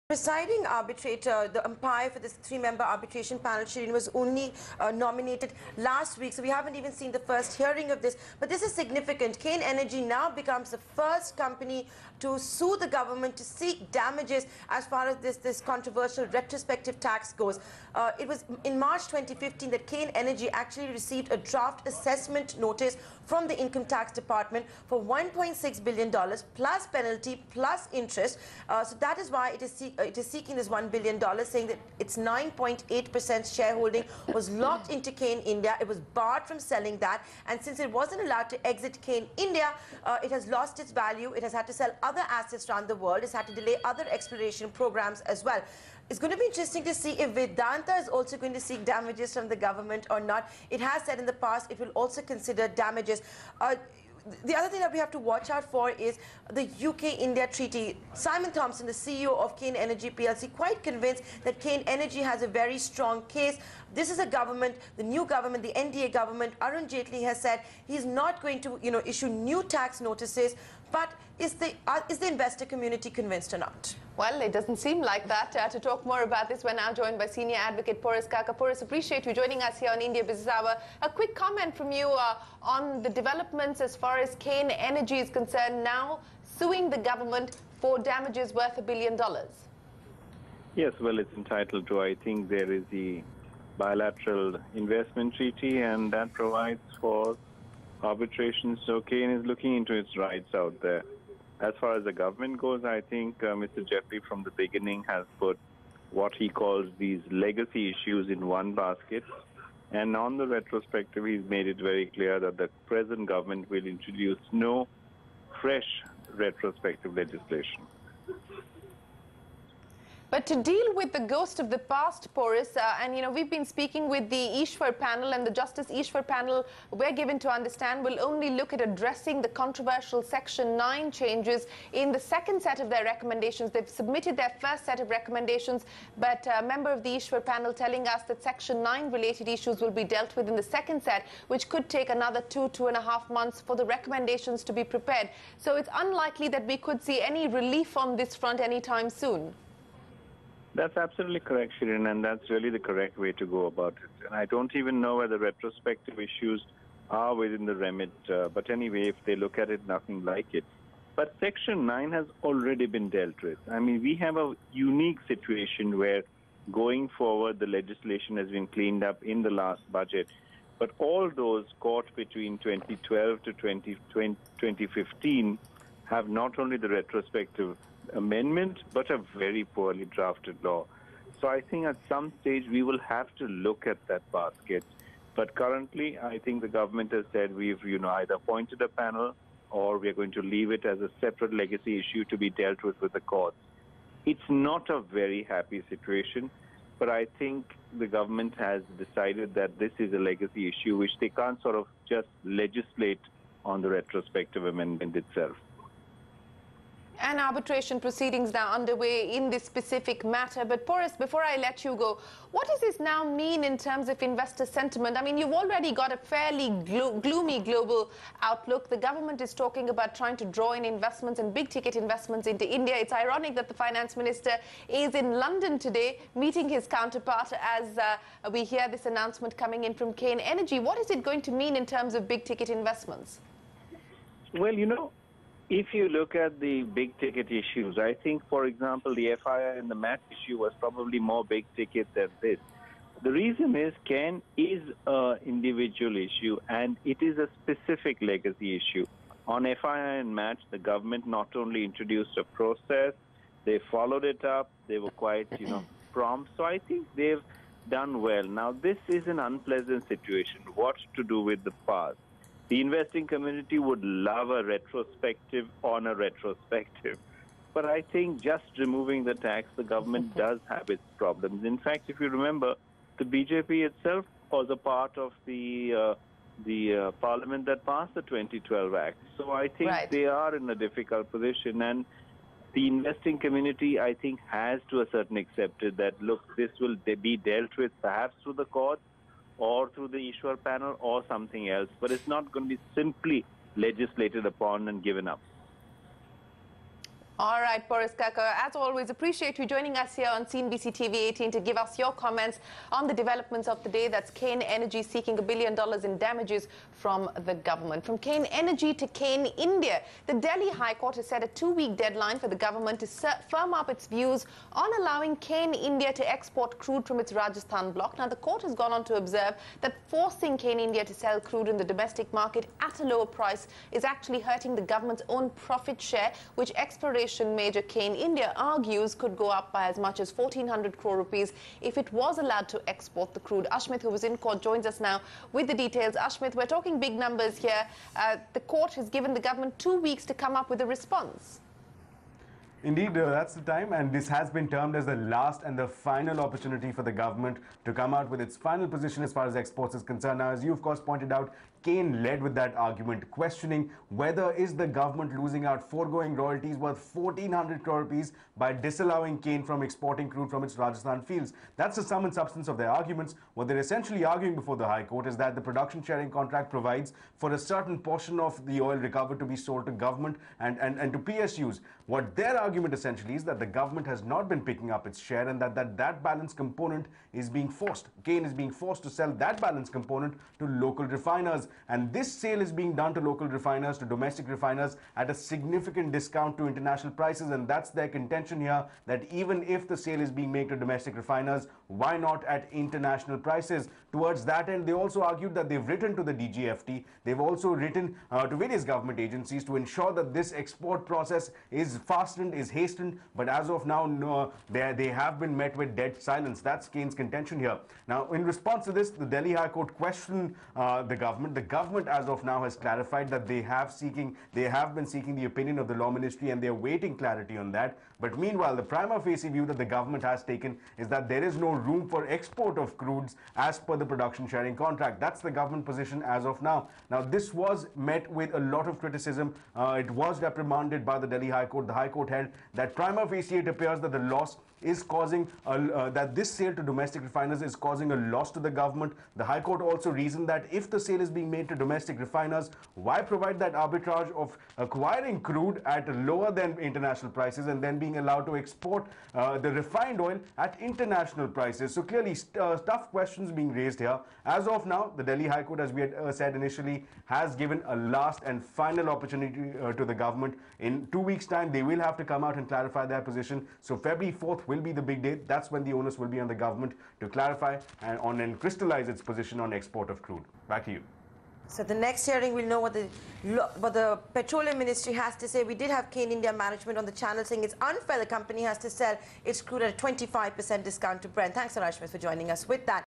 Cairn Energy's Retro Ghost Haunts Govt. After Co Sues For Damages Worth Billions
The retro ghost is back to haunt the government- Cairn Energy has sued the centre for damages to the tune of a billion dollars. The British company is seeking damages for losses it suffered in its Cairn India shareholding after the taxman slapped a 10,000 crore rupee notice using the retrospective amendment. And this comes at a time when Finance Minister Arun Jaitley is in the United Kingdom to meet his counterpart George Osborne.